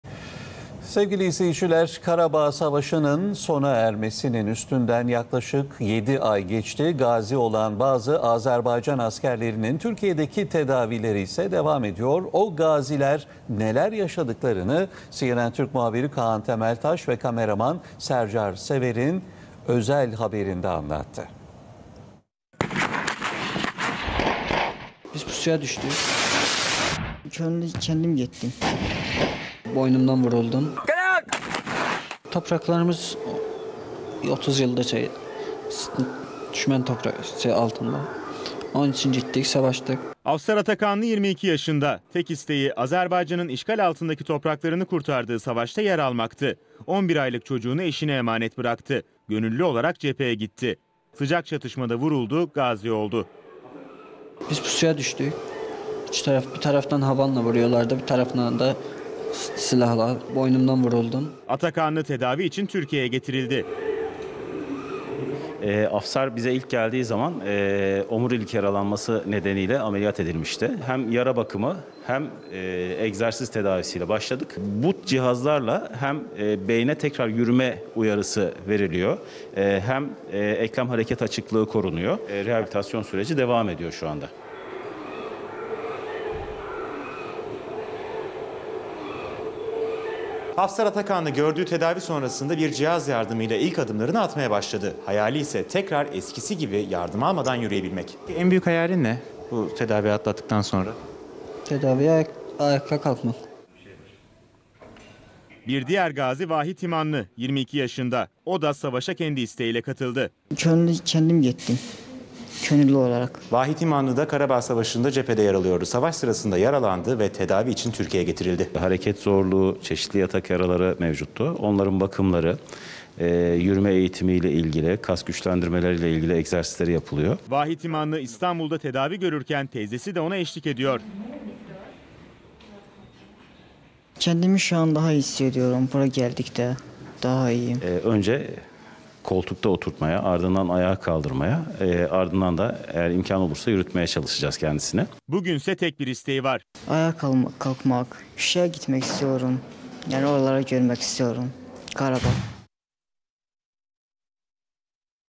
Gazi Azerbaycan askerleri tedavi için Türkiye'de
Karabağ Savaşı’nın sona ermesinin üstünden yaklaşık 7 ay geçti. Gazi olan bazı Azerbaycan askerlerinin Türkiye'deki tedavileriyse sürüyor. O gaziler neler yaşadıklarını, CNN TÜRK muhabiri Kaan Temeltaş ve kameraman Sercan Sever’in özel haberinde anlattı.

Afsar Atakanlı 22 yaşında. Tek isteği Azerbaycan’ın işgal altındaki topraklarını kurtardığı savaşta yer almaktı. 11 aylık çocuğunu eşine emanet bıraktı. Gönüllü olarak cepheye gitti. Sıcak çatışmada vuruldu, gazi oldu.

Atakanlı tedavi için Türkiye'ye getirildi.

Bir Diğer Gazi Vahid İmanlı, 22 yaşında. O da savaşa kendi isteğiyle katıldı. Vahid İmanlı İstanbul'da tedavi görürken teyzesi de ona eşlik ediyor.